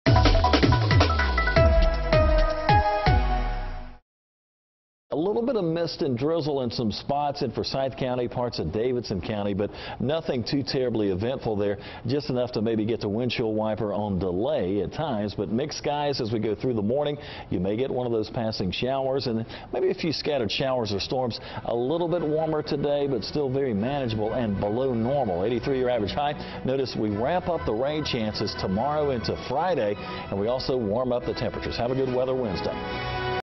Storms possible this Wednesday
Meteorologist Austin Caviness takes a look at your Wednesday forecast...